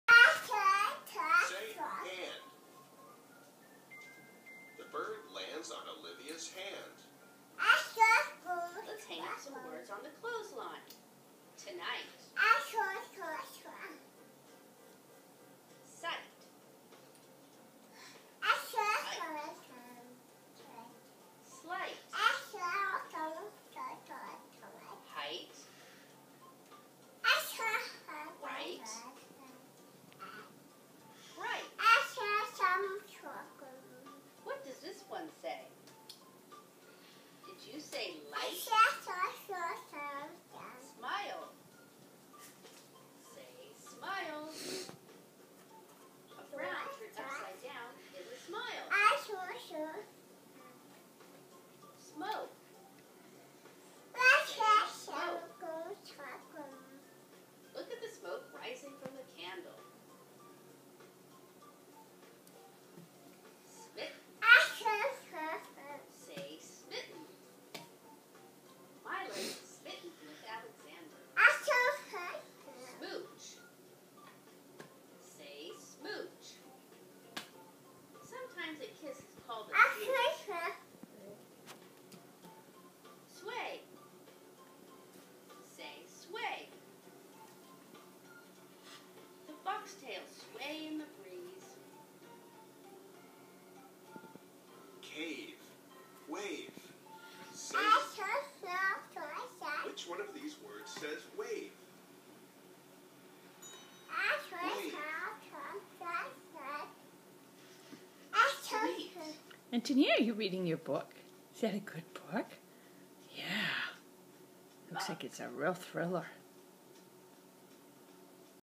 Antonia reading
Antonia is sharing her book - a real cliffhanger!